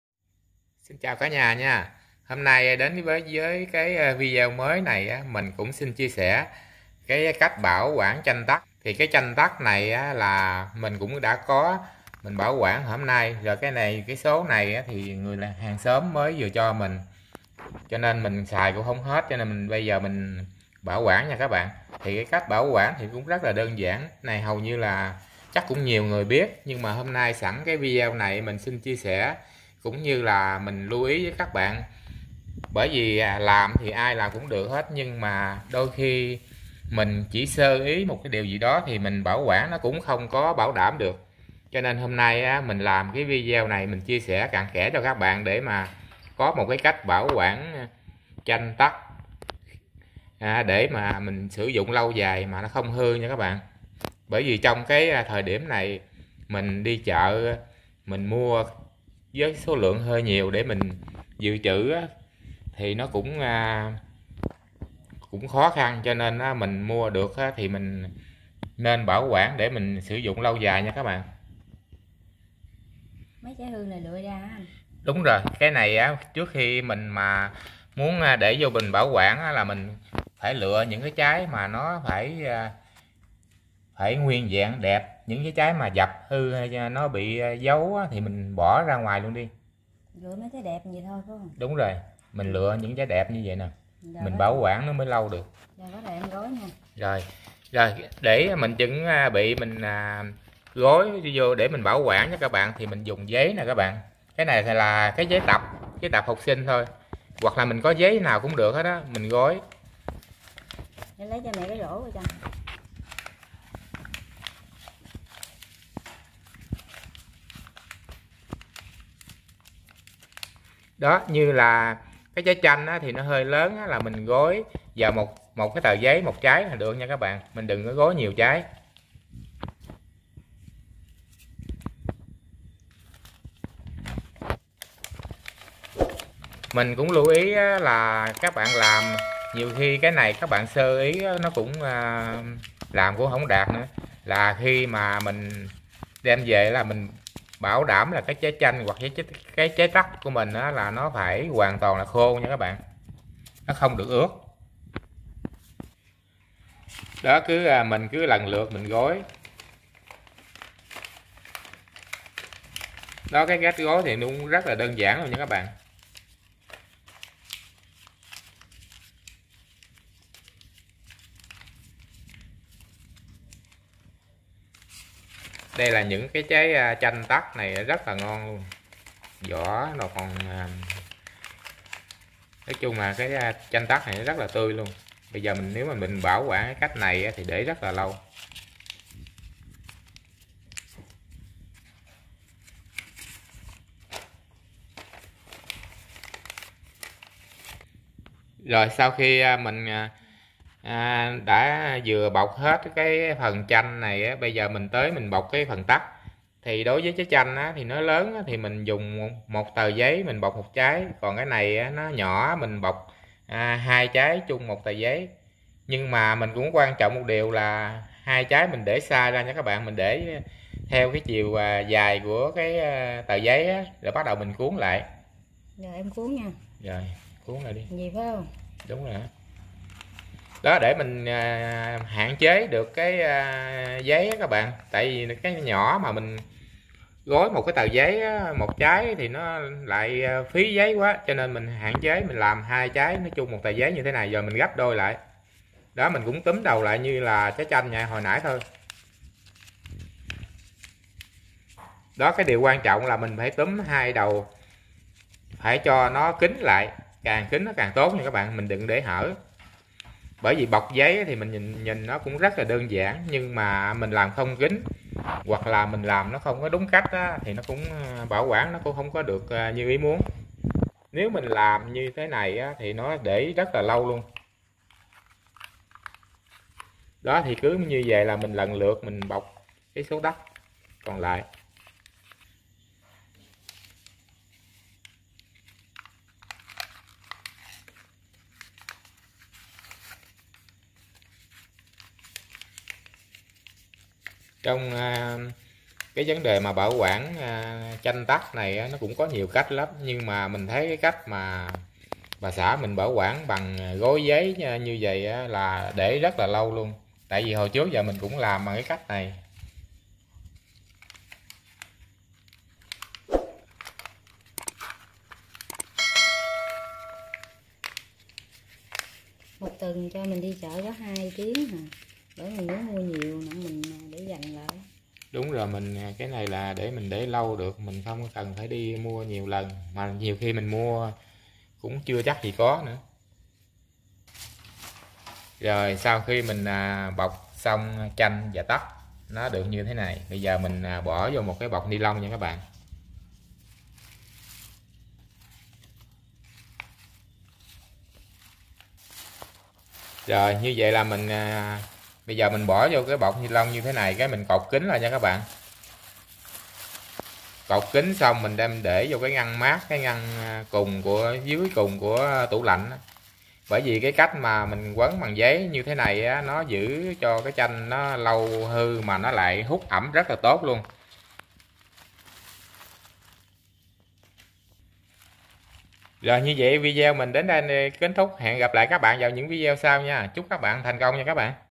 Cách bảo quản chanh tắc không hư How to preserve lemons without spoiling? Thảo 976
mời các bạn cùng xem cách bảo quản chanh tắc không hư